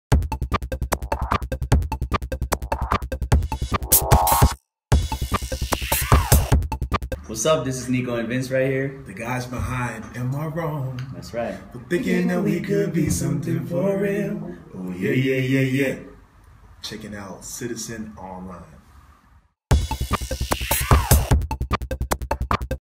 Nico and Vinz serenade Connect
How cool is it that we met Nico and Vinz? but wait... we even got ourselves a cool shout-out. Jealous yet?